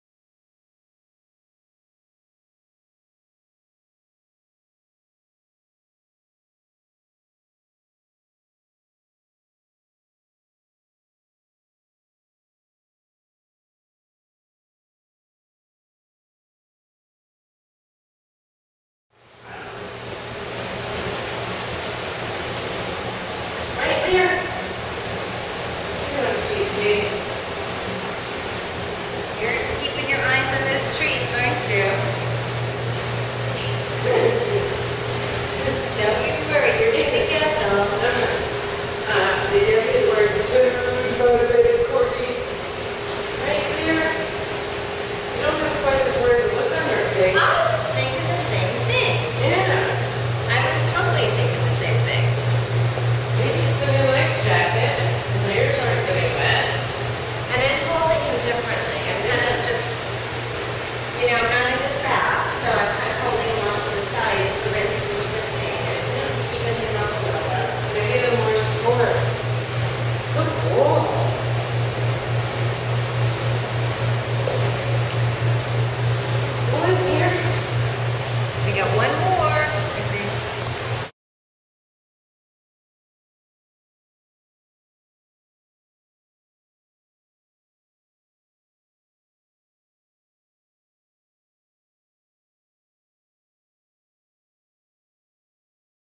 Corgi Llyr swimming
Llyr is a  13 1/2 year old Welsh Corgi. He has Degenerative Myelopathy. He is no longer using his back legs when he is swimming. Swimming is still helping his front legs stay strong. His attitude and spirit are still very good.